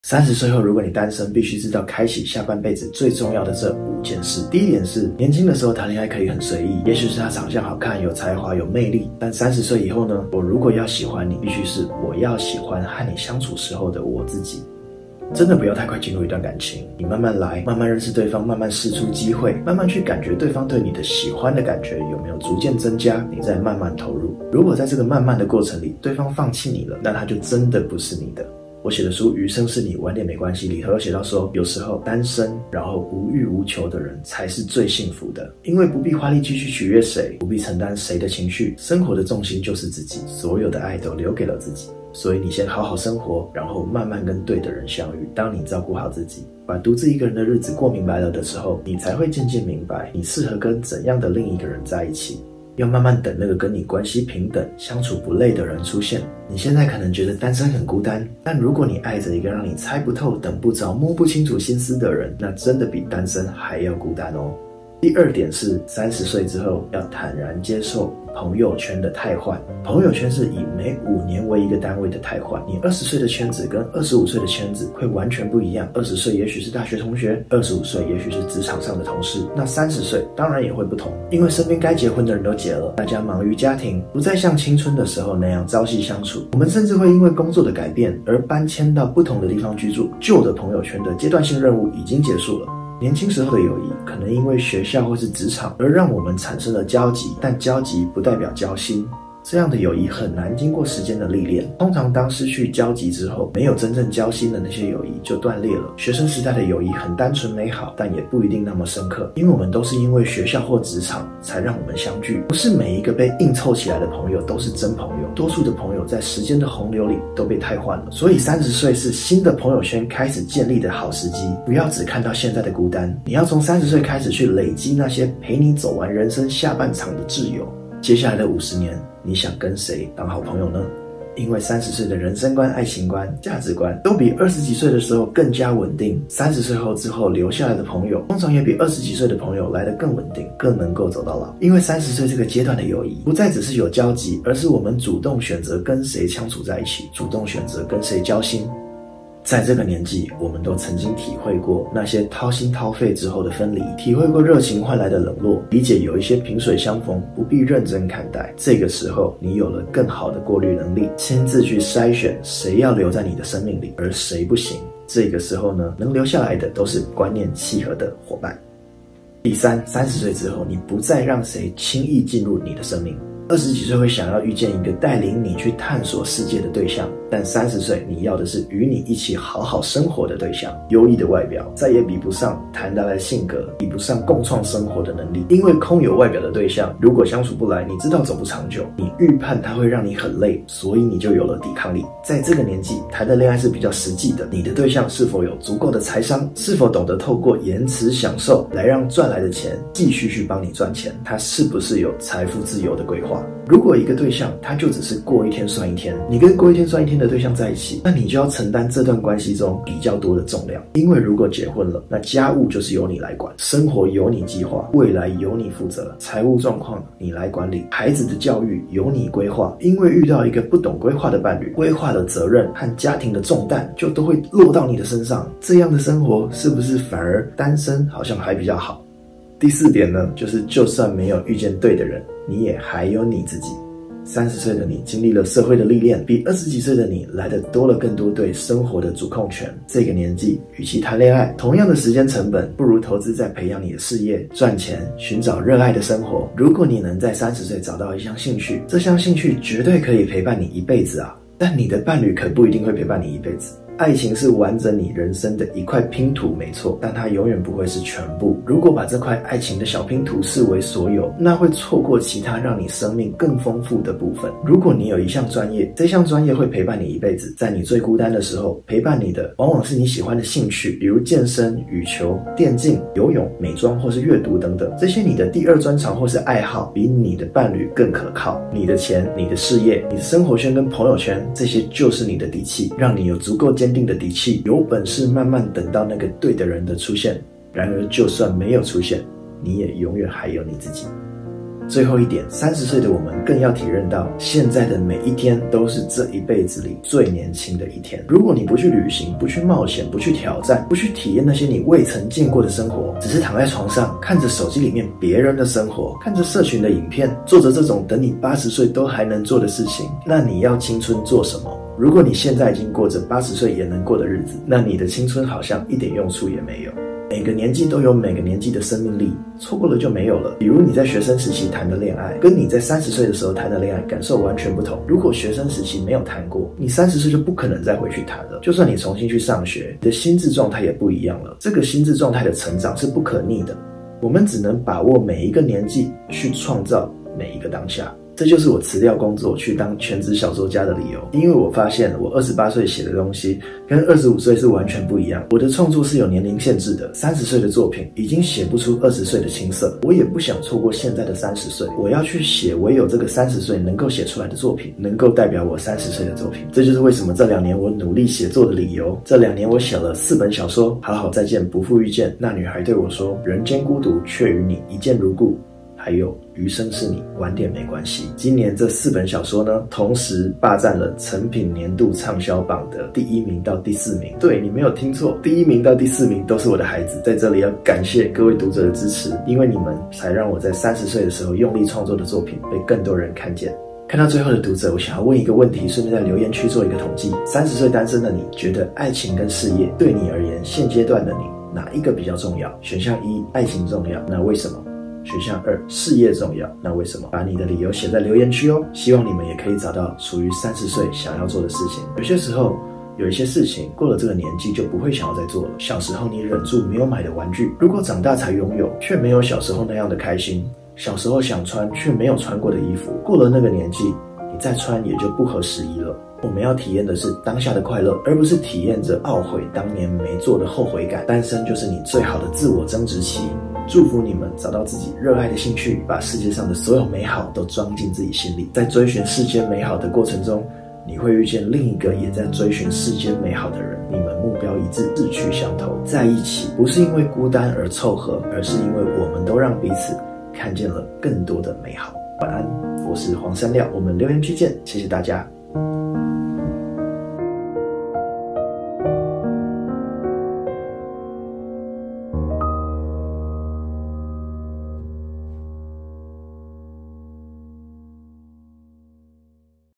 30歲單身 必須注意這5件事！單身是你最好的「自我增值期」
我寫的書《 餘生是你晚點沒關係 》

時間軸：
00:09 第一、我若要喜歡你，必須是我要喜歡跟你相處時的我自己
01:05 第二、坦然接受朋友圈的汰換
02:50 第三、不再讓誰輕易進入你的生命
03:49 第四、就算沒遇見對的人，你也還有你自己
04:53 第五、現在的每一天，都是這輩子最年輕的一天
05:59 山料寫的四本書

投票：06:26
事業和愛情，若只能擇一，你會選哪個？
1. 愛情重要
2. 事業重要

喜歡我的書的讀者，也可以到「誠品書店」現場閱讀，可以免費看完一整本。因為我有向出版社提出要求，希望書「不要封膜」，確保沒預算的讀者也可以閱讀！謝謝大家。